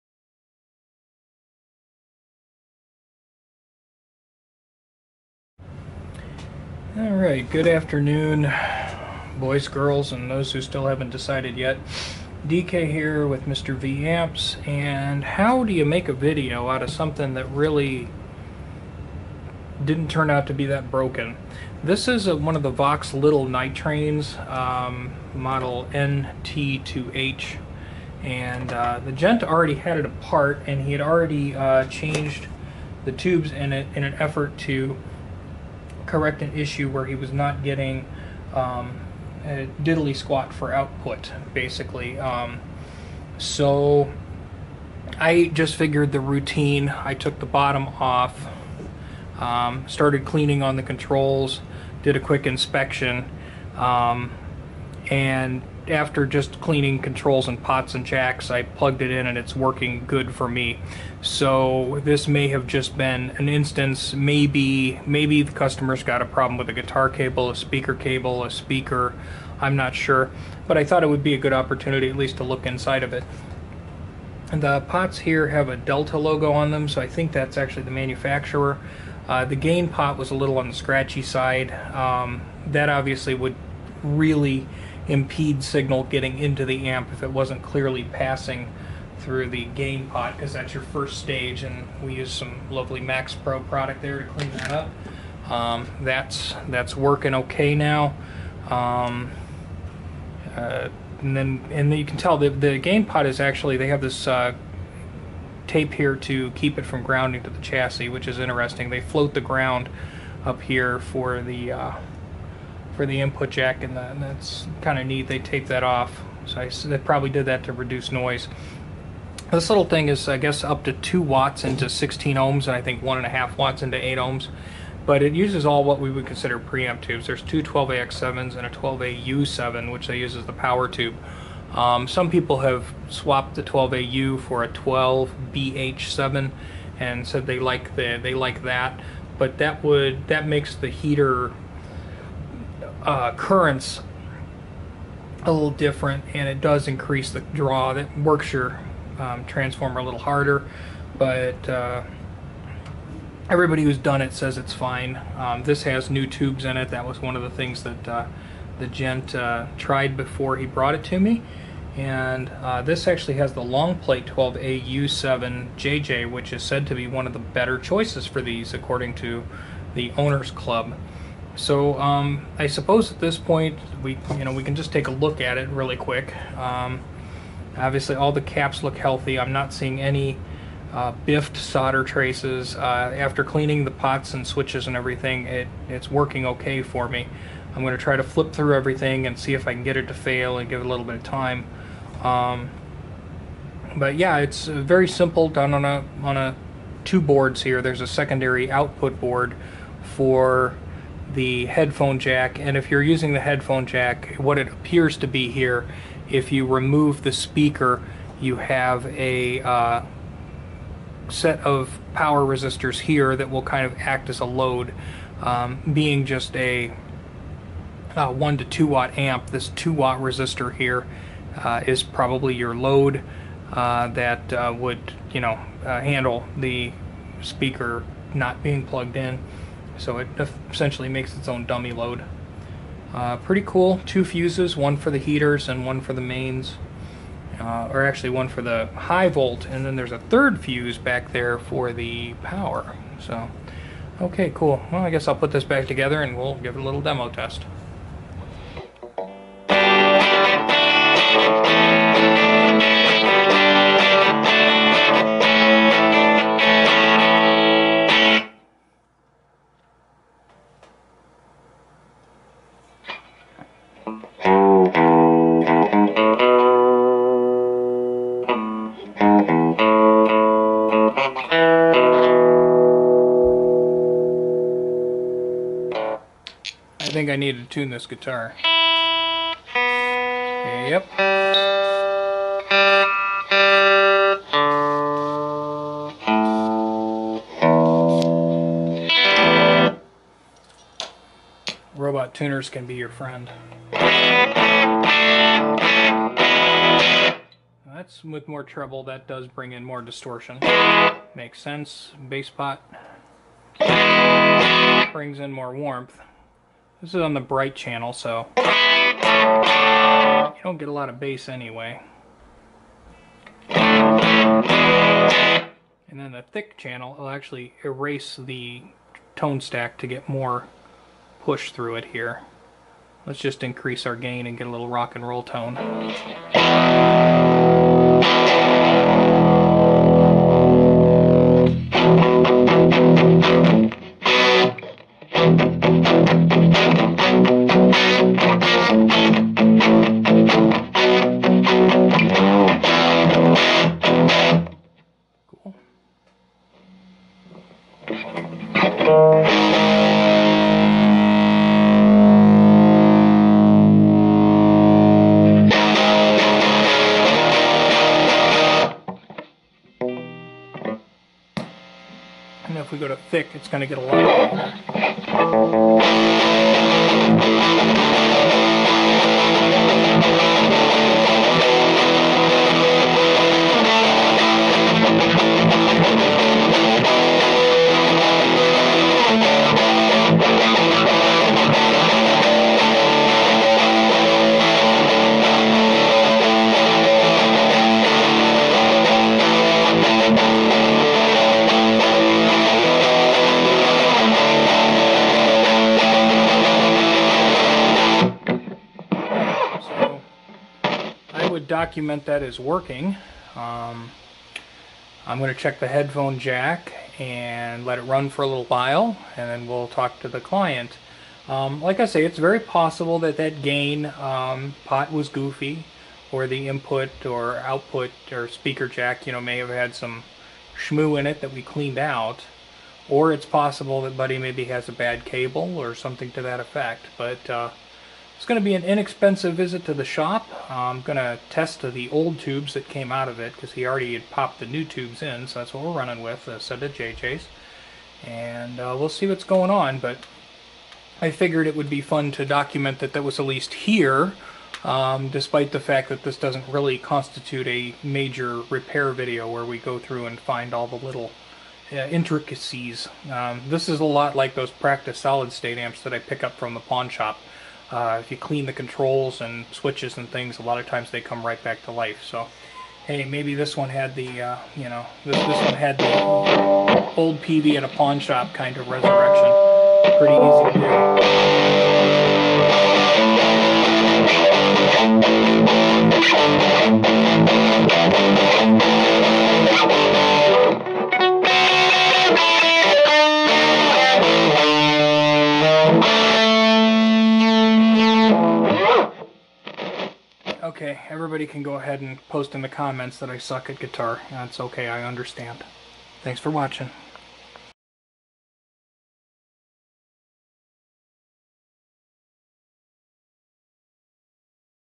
Vox Lil' Night Train Guitar Amp comes into the shop for service
This amp was a quick fix,  so sorry if you all were looking for diagnostics.   I thought it was cool enough to show it to you,  so that's at least something.   We'll be doing a big project here soon.